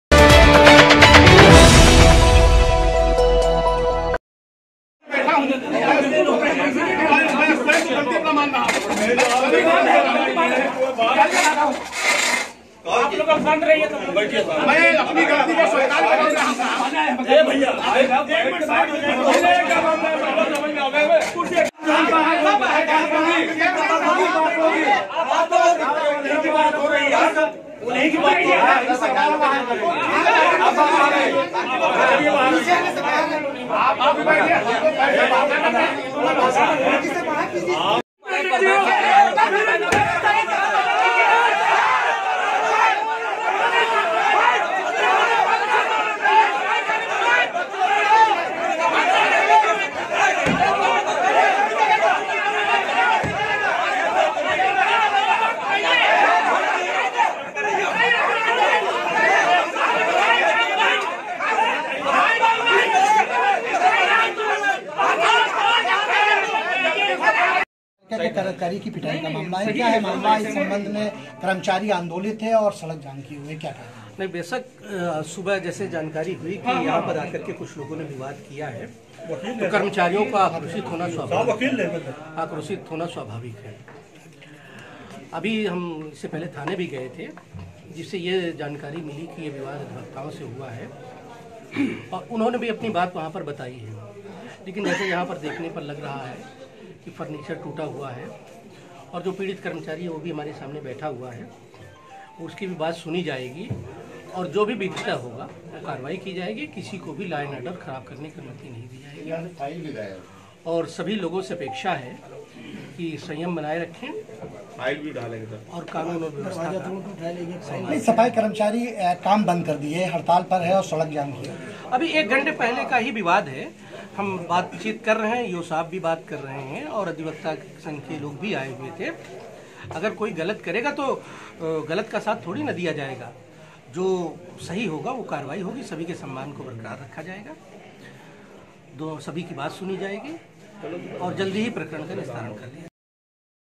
वकीलों की गुंडागर्दी पत्रकारों से अभद्रता
जनपद बहराइच में वकीलों की सरेआम गुंडई देखी गई है वकीलों ने पहले नगरपालिका में तैनात टैक्स अधिकारी को थप्पड़ मारा जिसके बाद विवाद काफी बढ़ गया/ देखते ही देखते नगर पालिका के कर्मचारियों और वकीलों के बीच गालीगलौज और झड़प होने लगी/ दो पक्षों में बवाल और झड़प की सूचना पाने के बाद कवरेज करने पहुँचे पत्रकारों से भी वकीलों ने जमकर हाथापाई की यही नही वकीलों ने पत्रकारों के शर्ट तक फाड़ डाले/ वकीलों की गुण्डई का आलम ये रहा कि नगरपालिका परिसर में घंटो अफरातफरी का माहौल कायम रहा/ इस मामले में जनपद के अधिकारियों ने  कहा कि जिस वकीलों ने पत्रकारों के साथ अभद्रता की है उनके खिलाफ कार्यवाही की जाएगी/